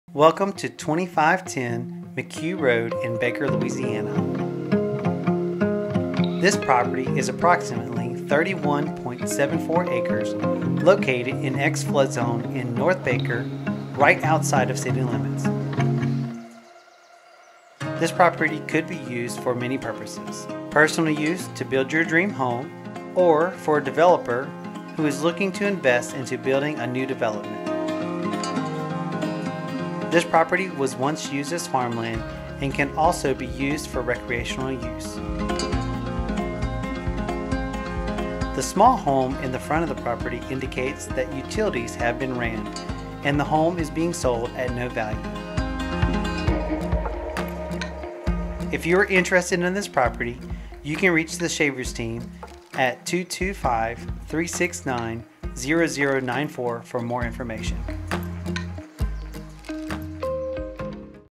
2510 McHugh Rd. Baker, LA 70714
30+ acres in North Baker. Great property for personal residence to build that forever home, or great property for an INVESTOR for creating a new development.